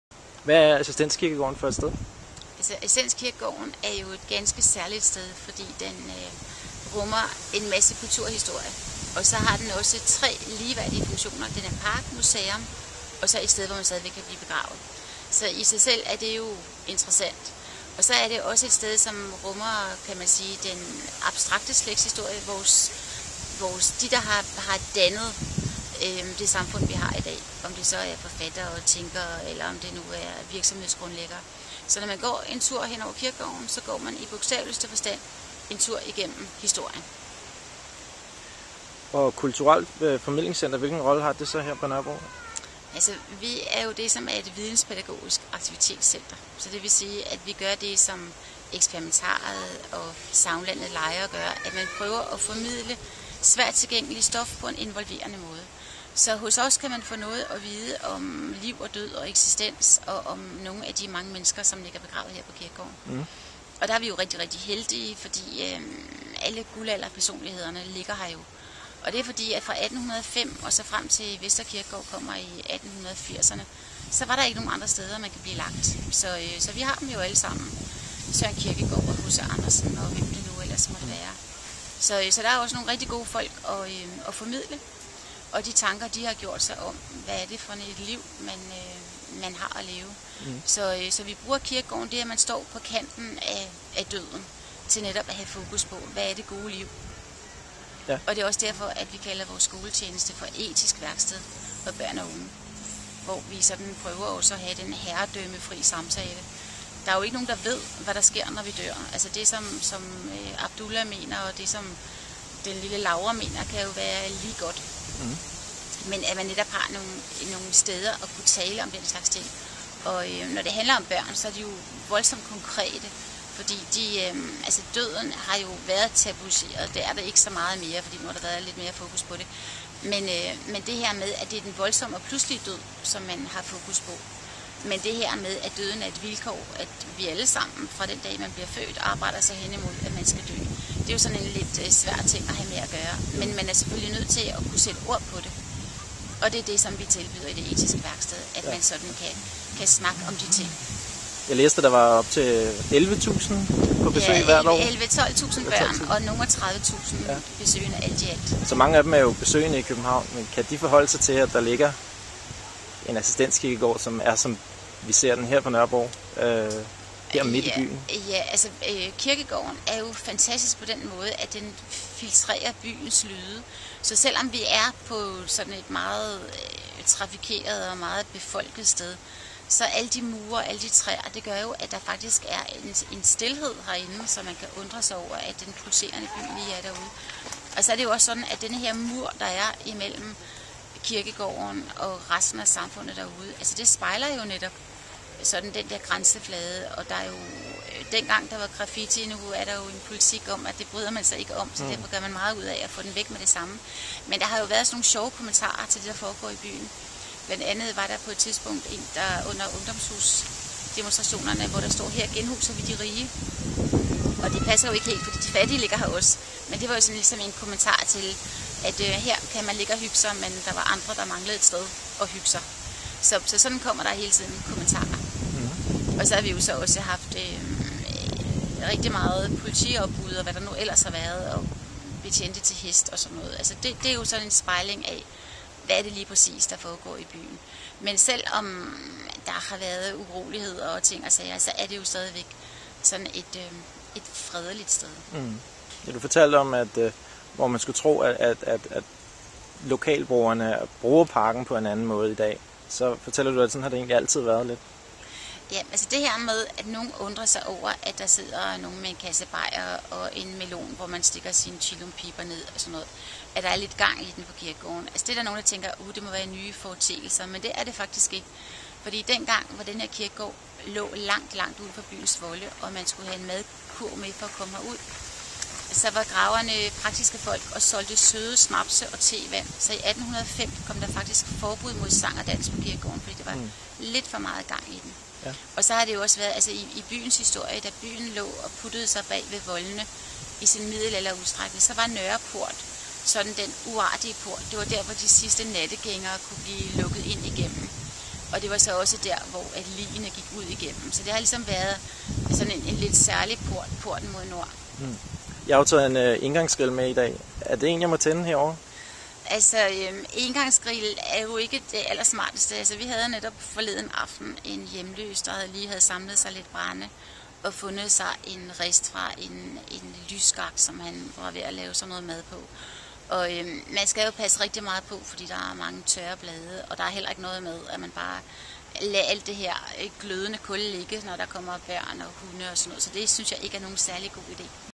Kulturcentret Assistens
Interview med Centerleder Gitte Lunding, antropolog